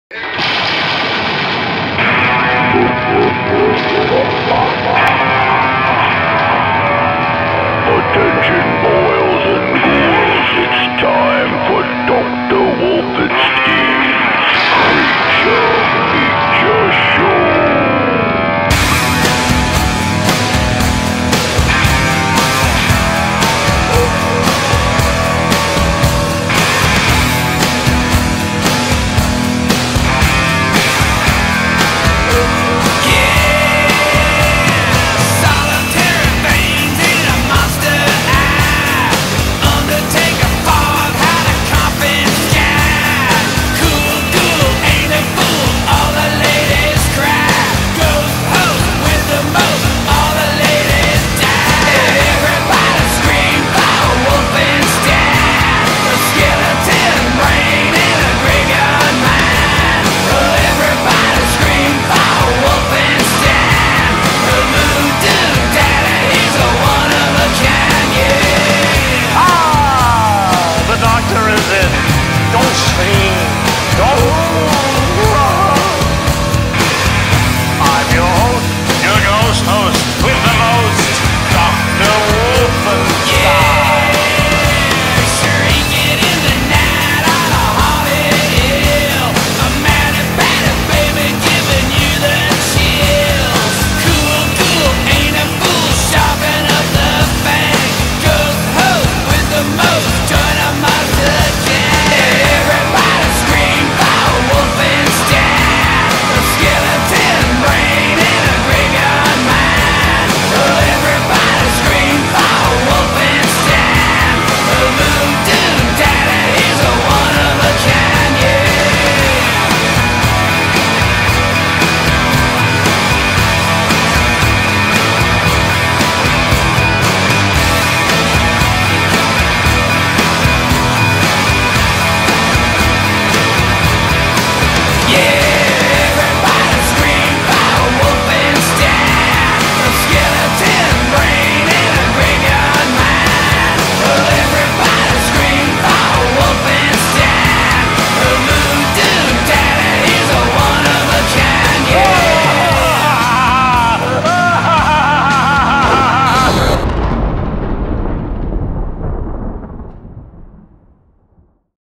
Black Haüs Creature Feature Show Reel
The last few weeks since moving to our new venue. Featuring vampire Masquerade, cenobite and the Boulet Brothers Dragula Titans Viewing party.
at the Renaissance Theater Company Orlando